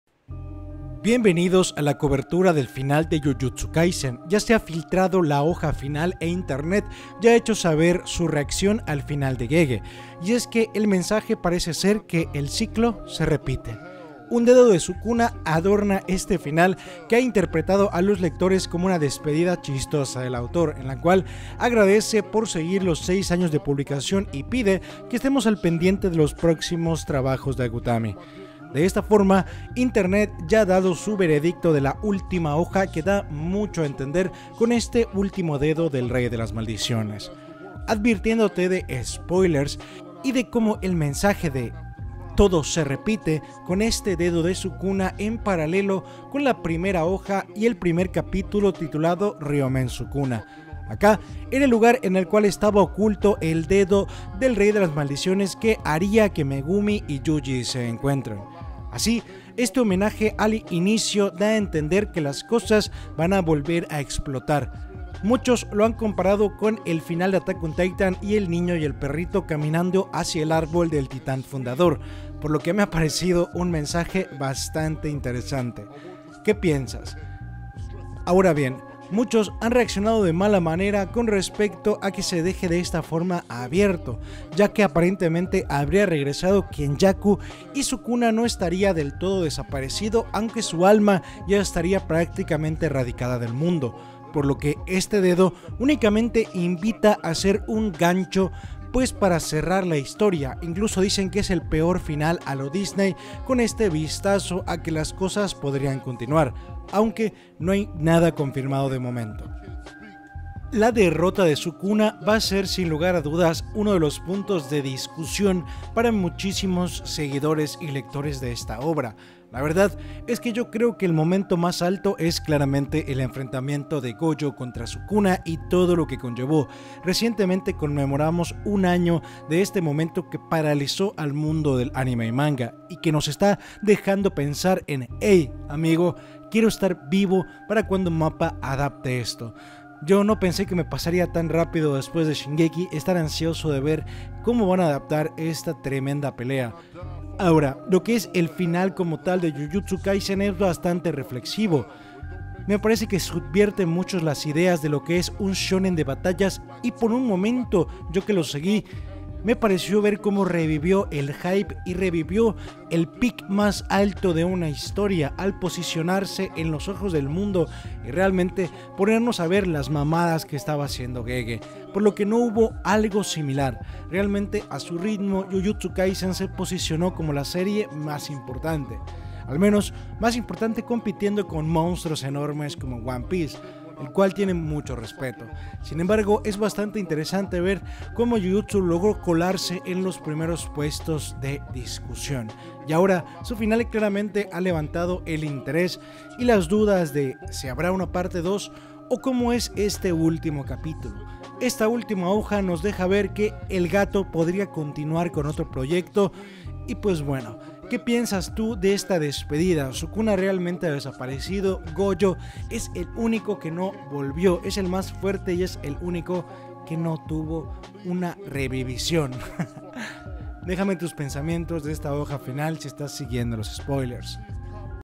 LA HOJA FINAL de JUJUTSU KAISEN : Manga 271 SPOILERS
JujutsuKaisen jujutsuKaisen271 sukuna jjk271 se ha filtrado la página final así como el mensaje de despedida del gato e internet reacciona a la hoja final que nos da a entender que "el ciclo se repite".